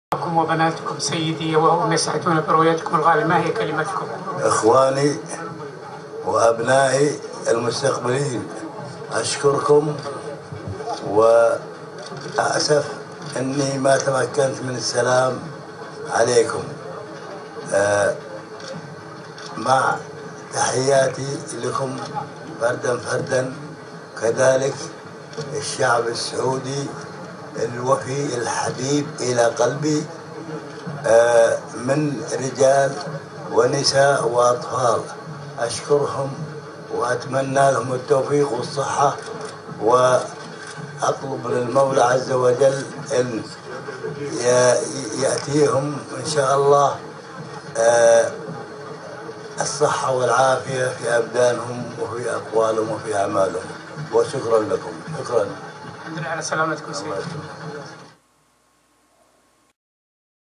The Sauds- Khadim Haramain Sharafain, Abdulla bin Abdul Aziz in Riyadh airport.
The Saudi King, His Majesty, Abdulla bin Abdul Aziz Al Saud arrives from New York in Riyadh and greets his countrymen.